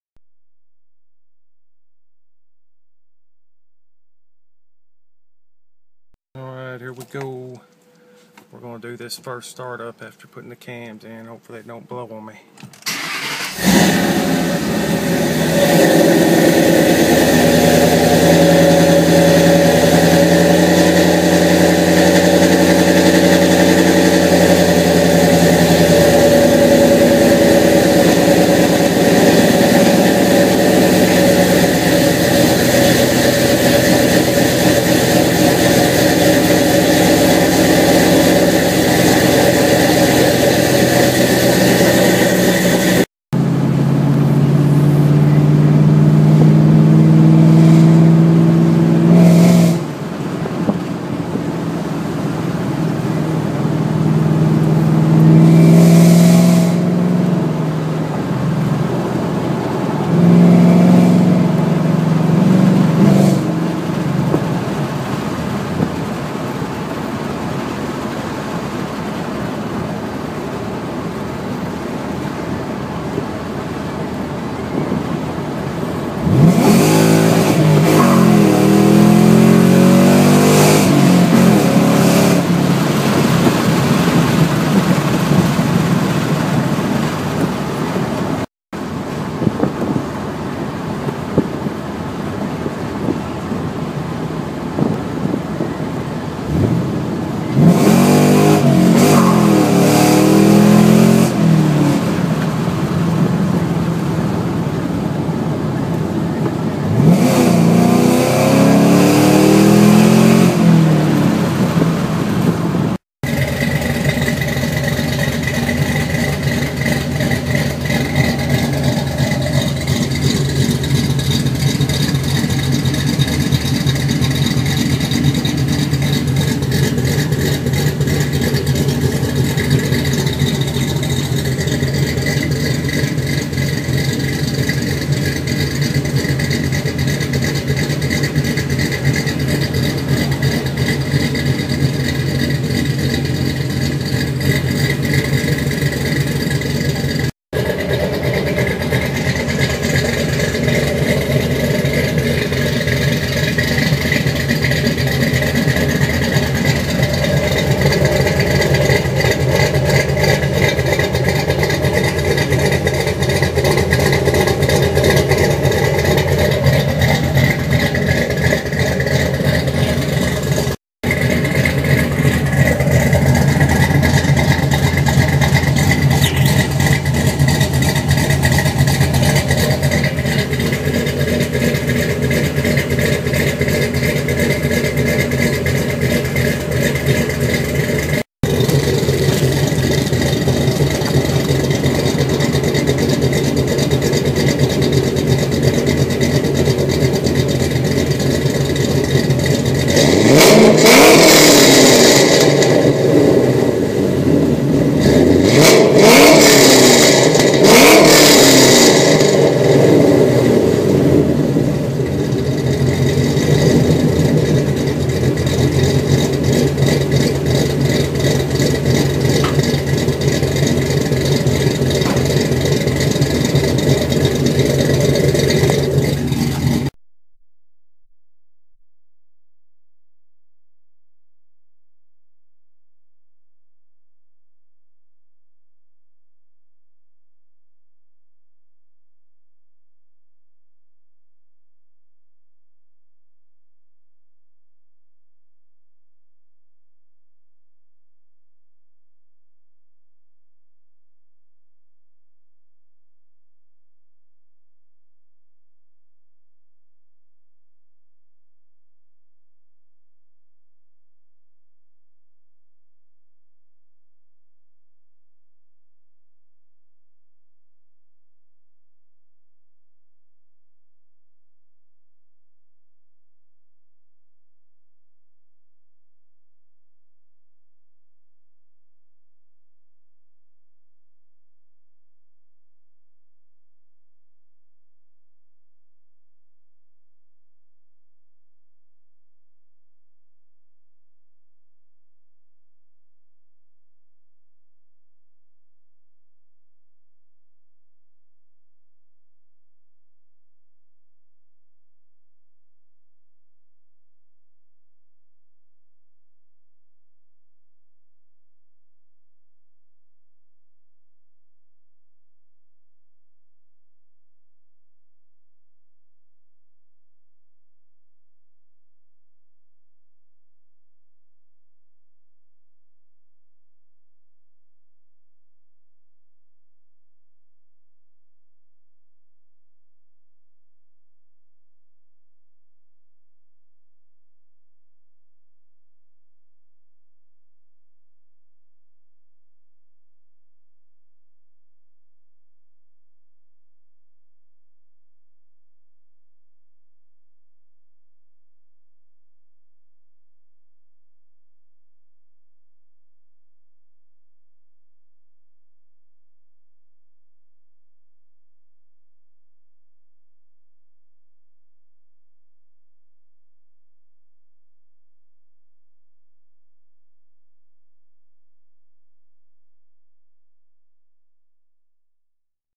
05 06 07 Mustang GT Detroit Rocker Cam Camshaft install shor
Installed Detriot rocker camshafts, electric water pump and delete plates. Start up video, test drive and that sweet idle!!